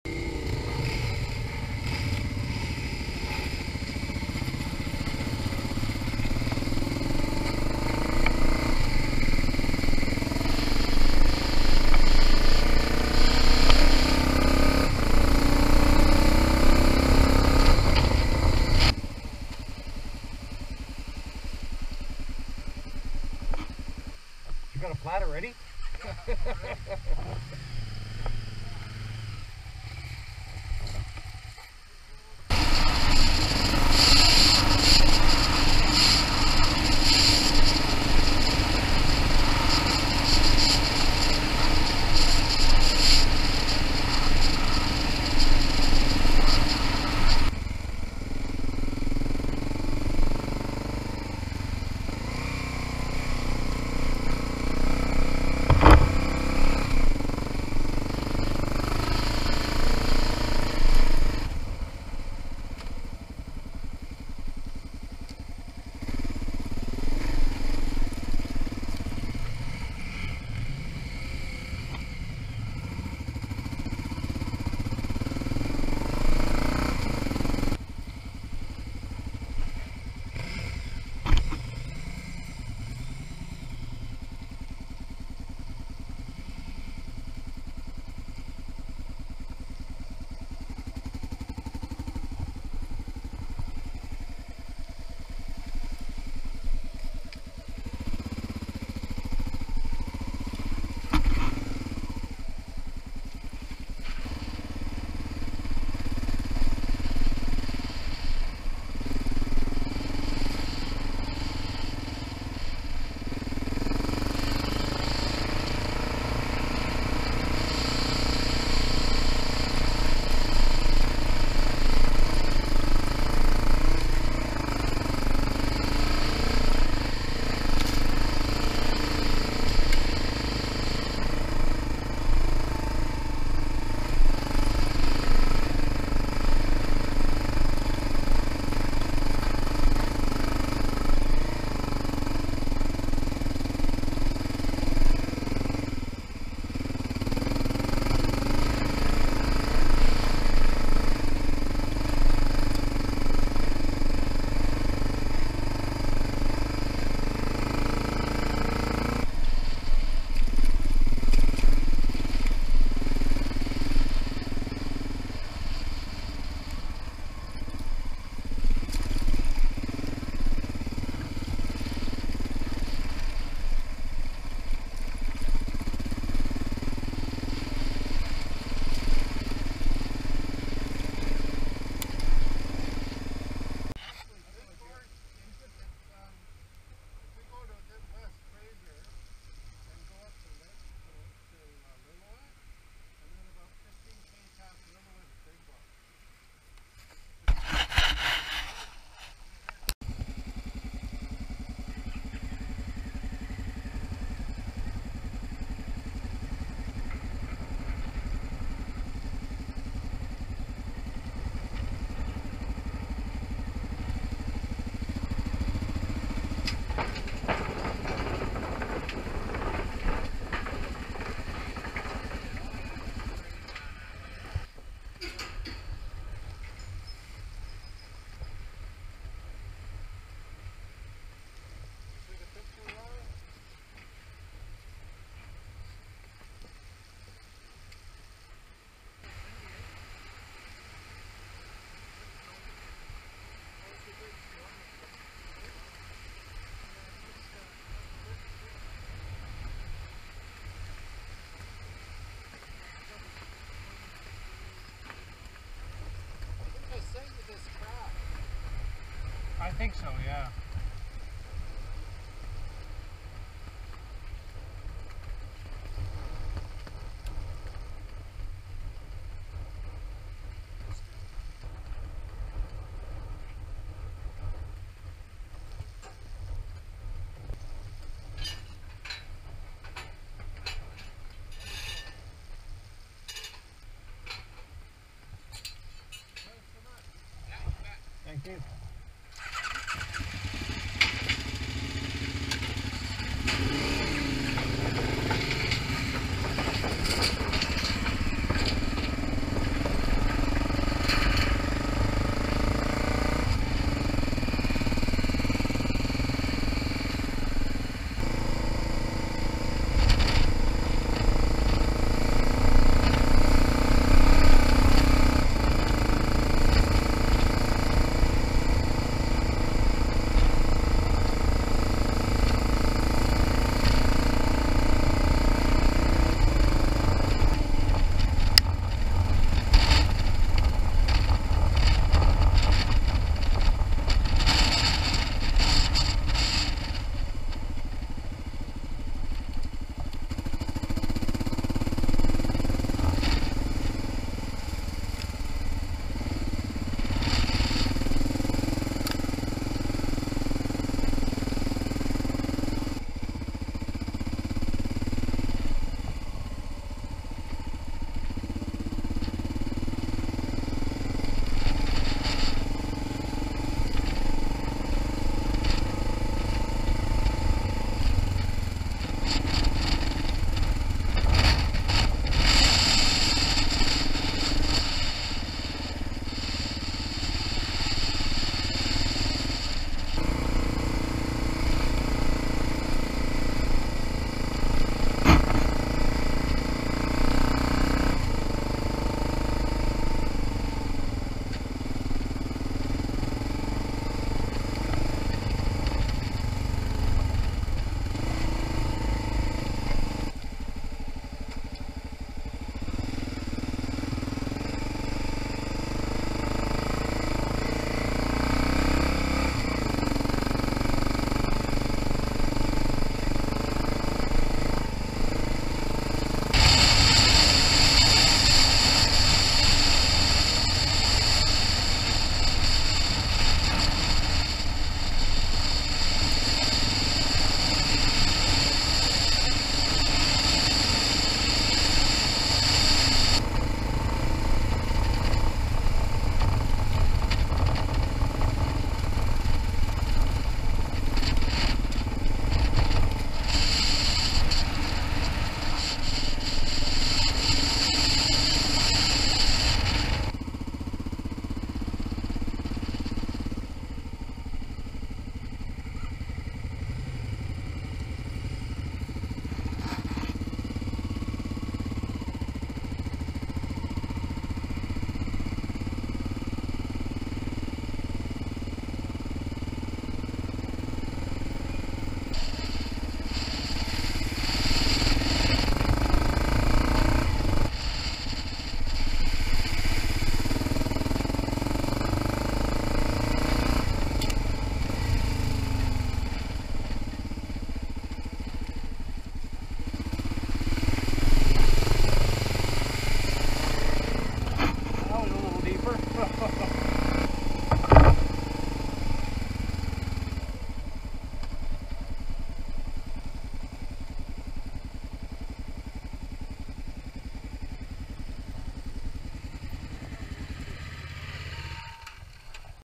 2018 09 08 East Harrison and Lytton
Riding Harrison East FSR from Harrison Hot Springs to Boston Bar and 2-track from Lytton back to Boston Bar.